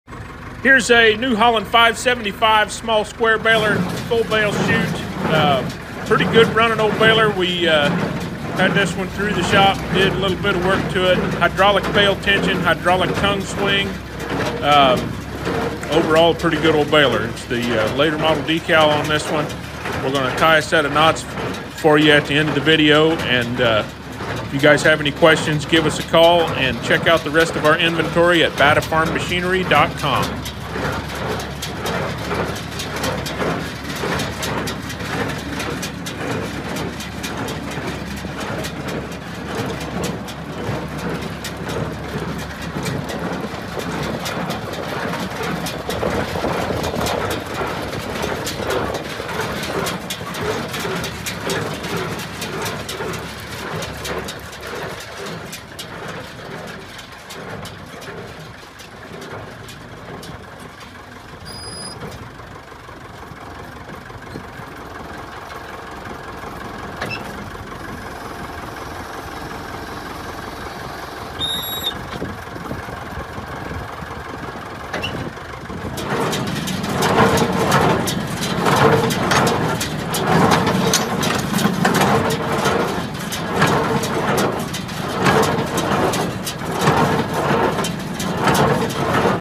NEW HOLLAND 575 For Sale
Twine tie, hydraulic bale tension and tongue swing, full bale chute. Watch the video for more information.  Fresh Square Balers